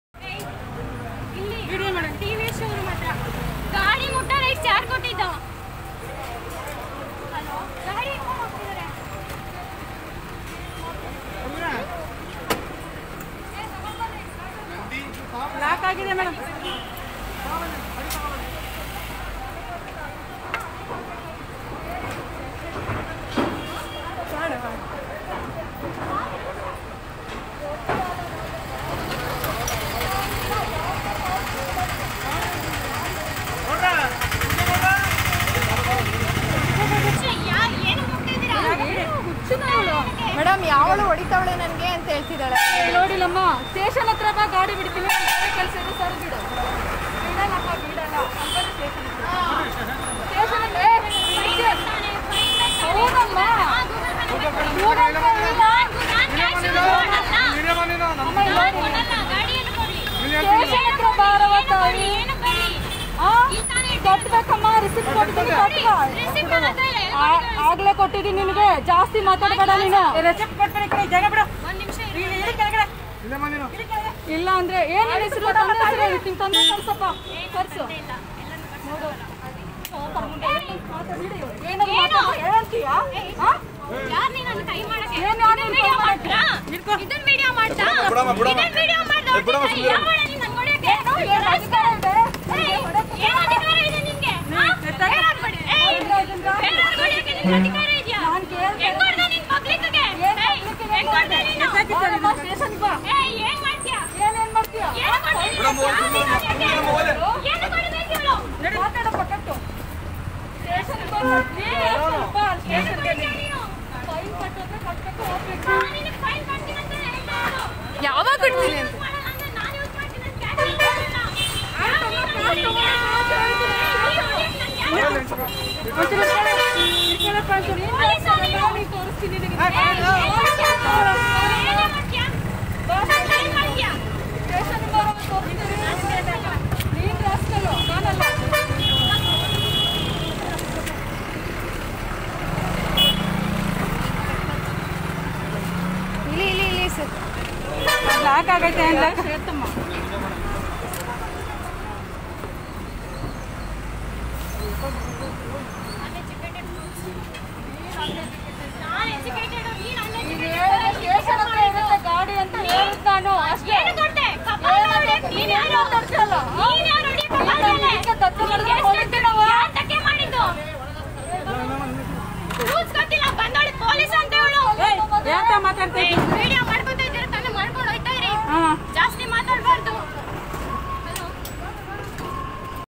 ಹೆಲ್ಮೆಟ್ ಹಾಕದಿದ್ದ ವಿಚಾರಕ್ಕೆ ಪೊಲೀಸರಿಂದ ಯುವತಿಗೆ ಕಪಾಳಮೋಕ್ಷ
police # Mandya viral